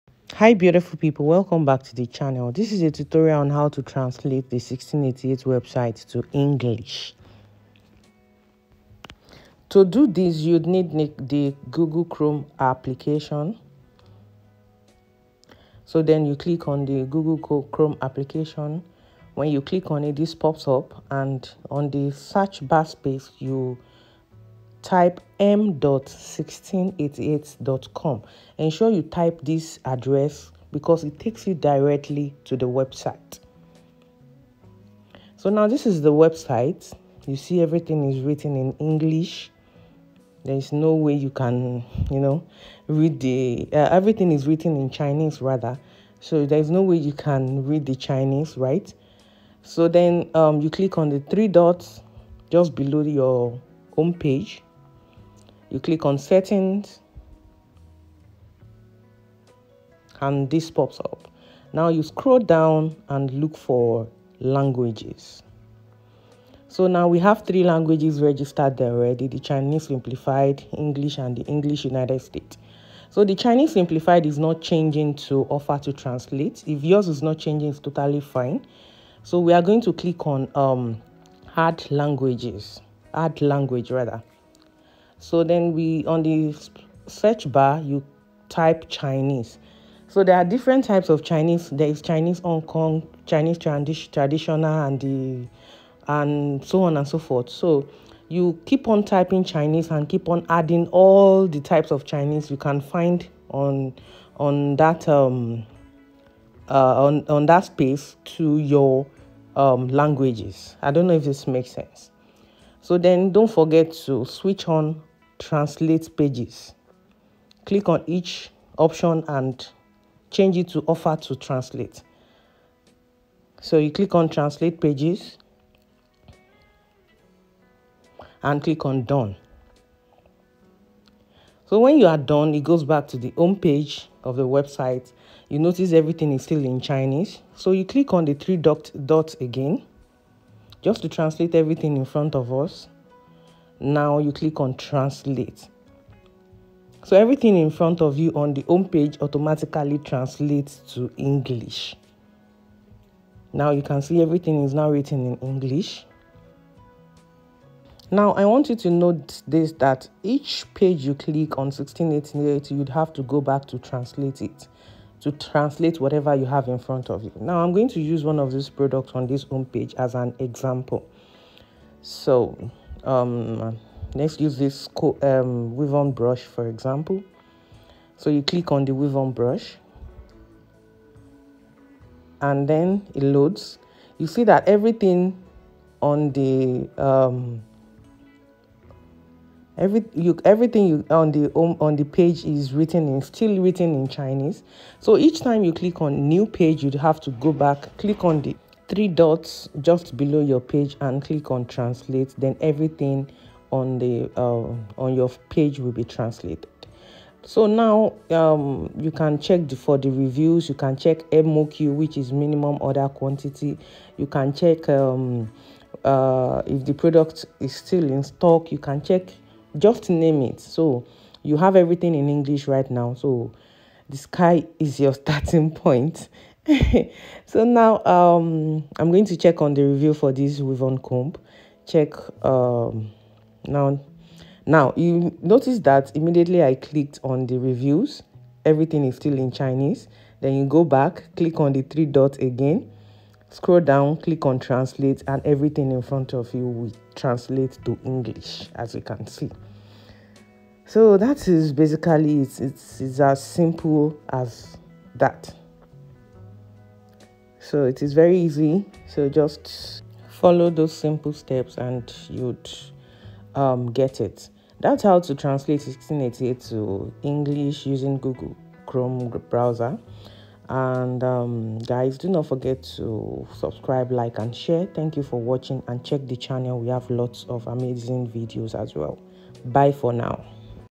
How To Translate 1688 Website To English | 1688.com English Translation
In today’s tutorial I’m going to be showing you how to translate 1688.com from chinese language to english for easy shopping and clear understand of the website
Ensure to watch till the end to avoid missing important steps.

Timestamps:
00:00 How To Translate 1688 Website To English | 1688.com English Translation
00:03 Introduction to translation of 1688 from Chinese to English
00:14 Step 1 to translating 1688.com
00:52 Step 2 to translating 1688.com
02:11 Step 3 to translating 1688.com
2:57 Step 4 to translating 1688.com